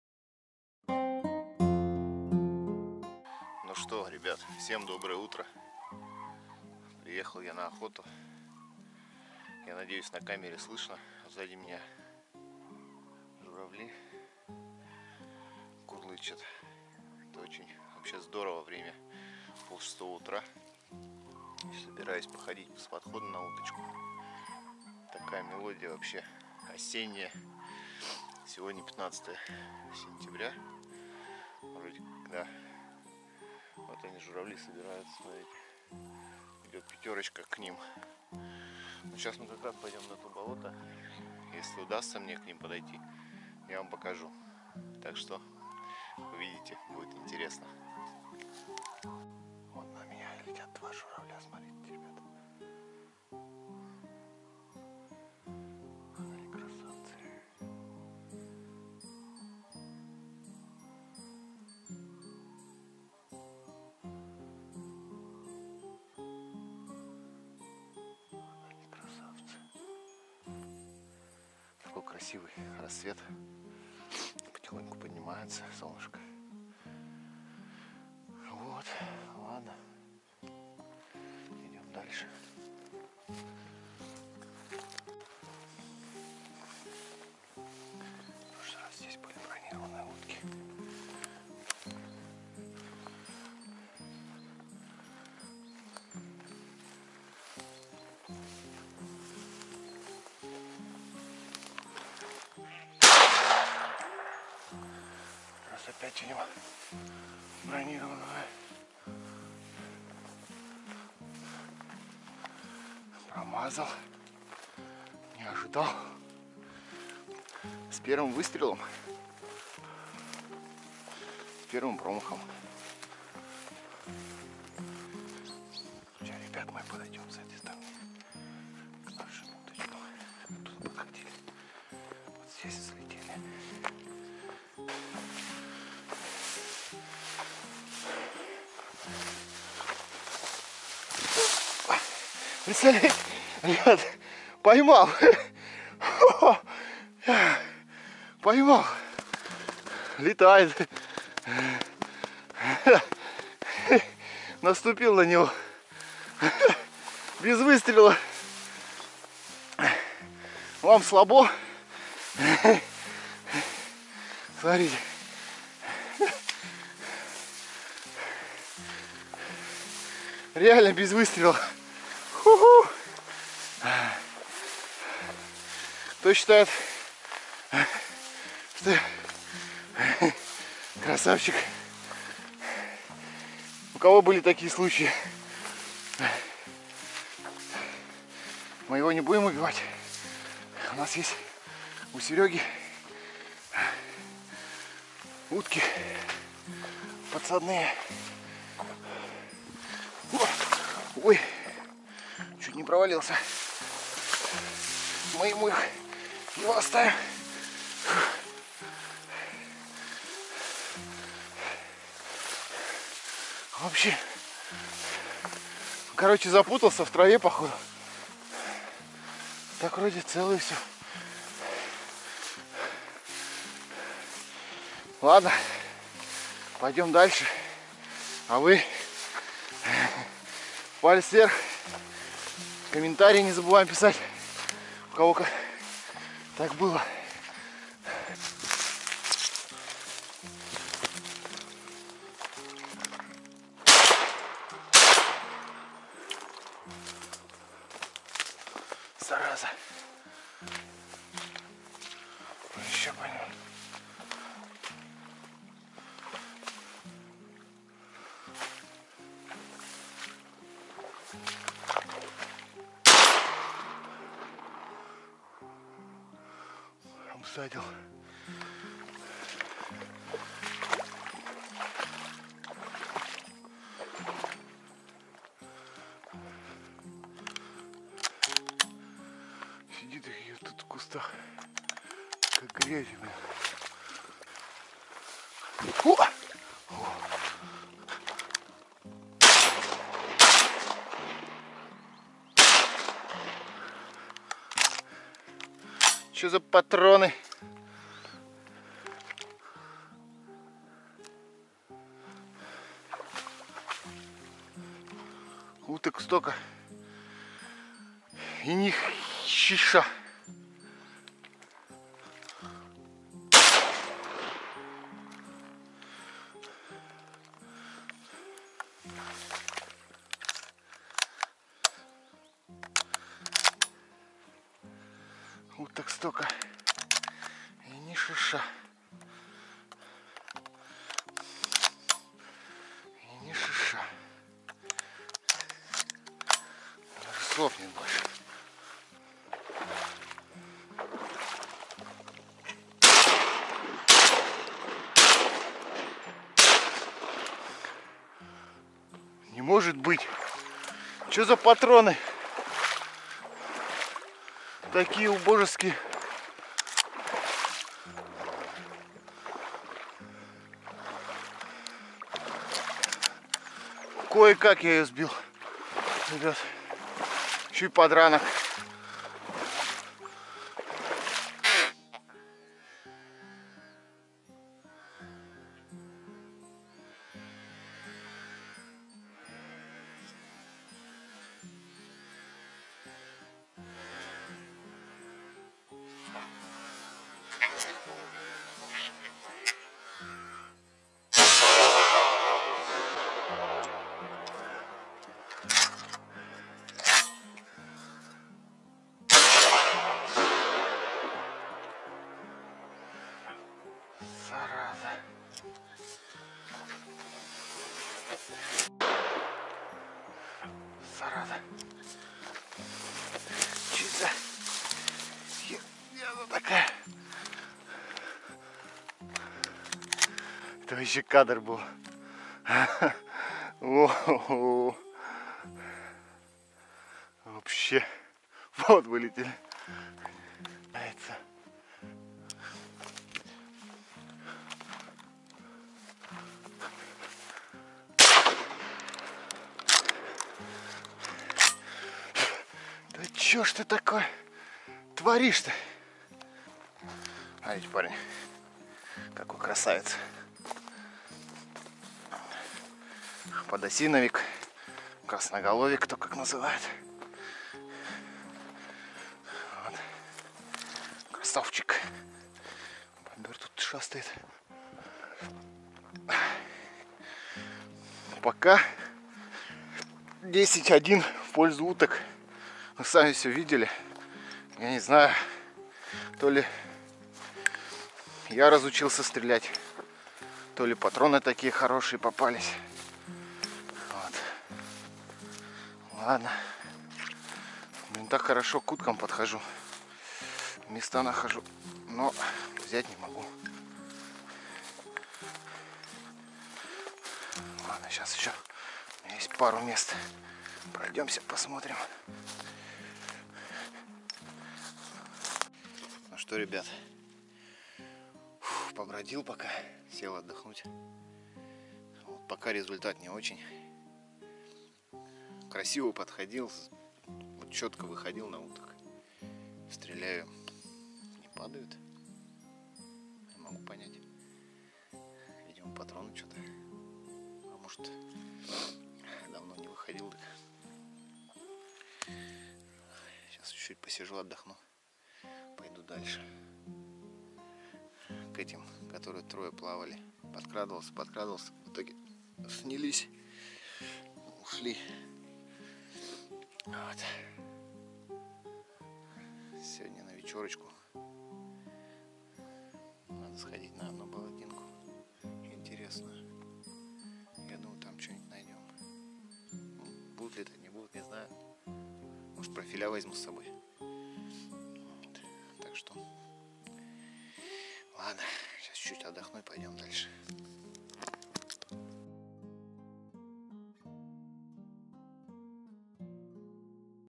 ОХОТА НА УТКУ С ПОДХОДА DUCK HUNTING 2018 . МОРЕ УТКИ ! САМЫЙ НЕ УДАЧНЫЙ ДЕНЬ ЖЕСТЬ !!!!!
ВСЕМ ПРИВЕТ !
ВОТ И ГОТОВО НОВОЕ ВИДЕО С ОХОТЫ НА УТКУ .
СКАЖУ СРАЗУ , УТКИ БЫЛО МНОГО ))) НО ДЕНЬ БЫЛ НЕ МОЙ УВЫ. ЗА ТО МНЕ УДАЛОСЬ ПОЙМАТЬ УТКУ РУКАМИ БЕЗ ВЫСТРЕЛА . В ОБЩИМ СМОТРИТЕ САМИ !!!